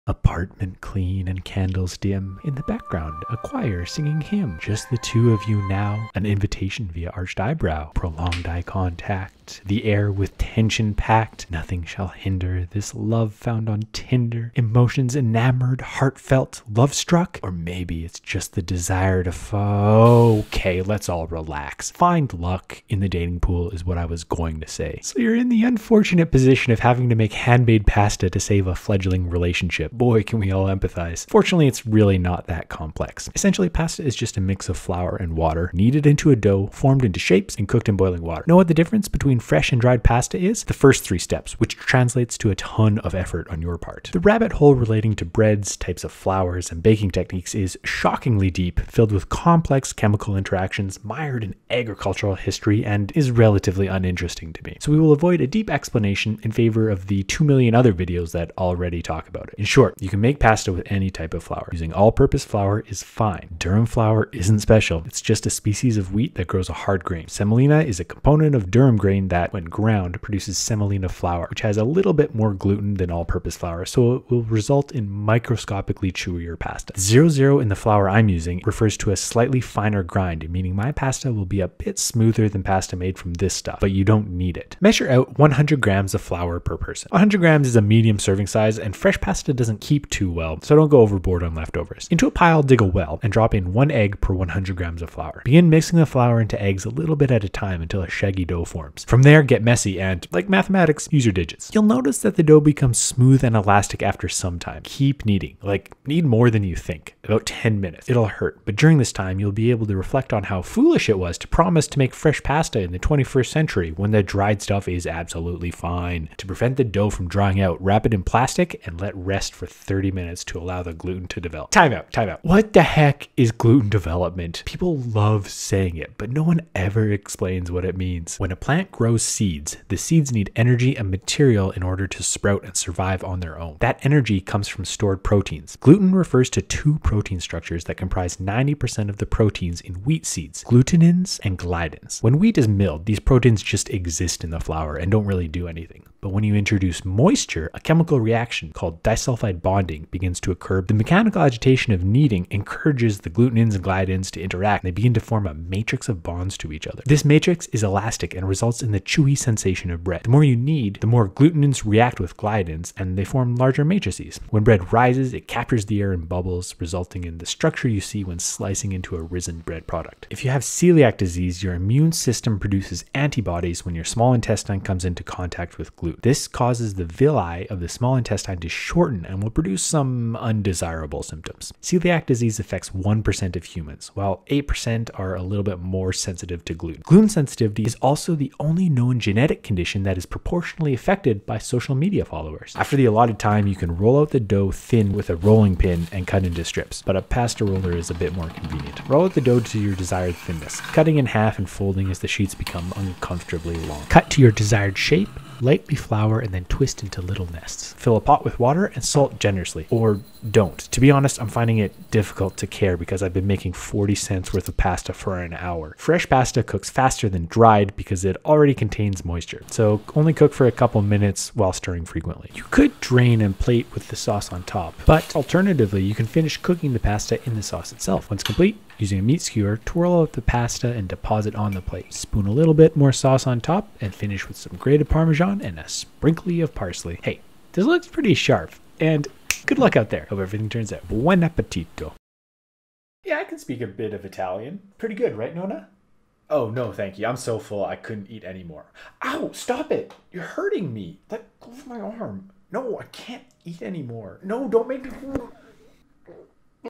Panicked Hand Made Pasta
Pennies worth of product for dollars worth of work.

00:00 A poem
00:27 The predicament
00:44 Bread Science
01:25 Recipe
02:01 What is Gluten Development?
03:13 Pasta Rolling
03:48 Plating
03:58 Good Luck